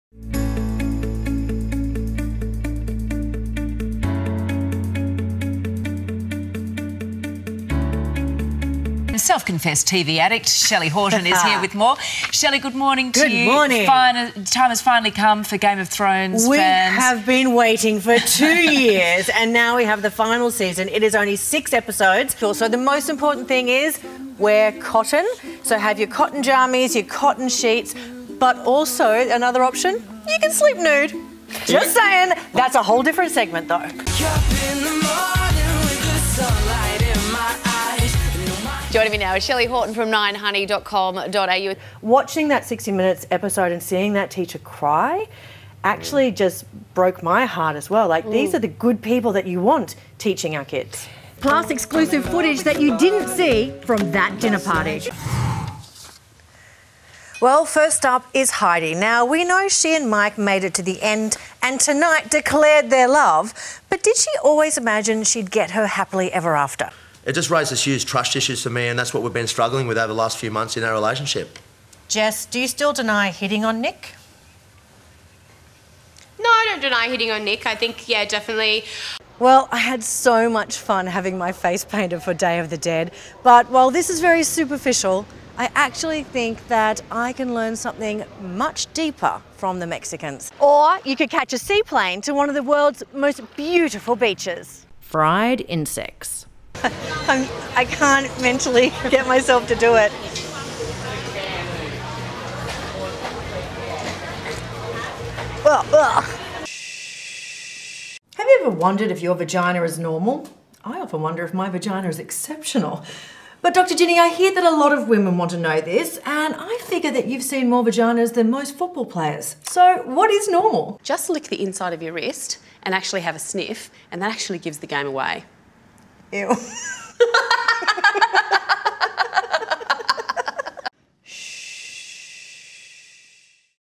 Shelly Horton's Sizzle Reel 2019
A brief look at Shelly Horton's TV and video work over the past year or two.